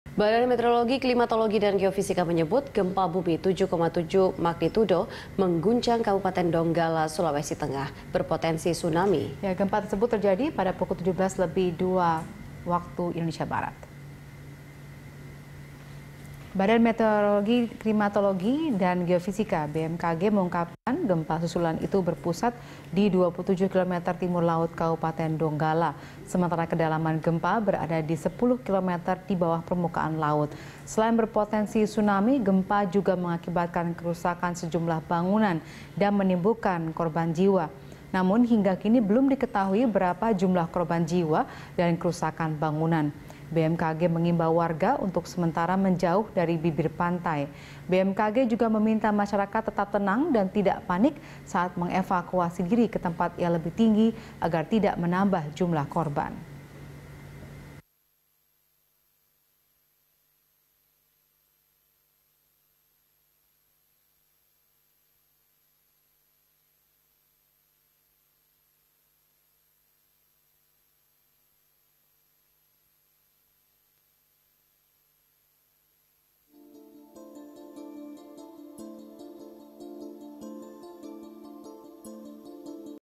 Gempa di Donggala Berpotensi Tsunami
BMKG menyebut gempa bumi 7,7 magnitudo mengguncang Kabupaten Donggala, Sulawesi Tengah berpotensi tsunami. Gempa tersebut terjadi pada pukul 17.02 WITA.

Facebook.com/BeritaSatuTV
Youtube.com/BeritaSatu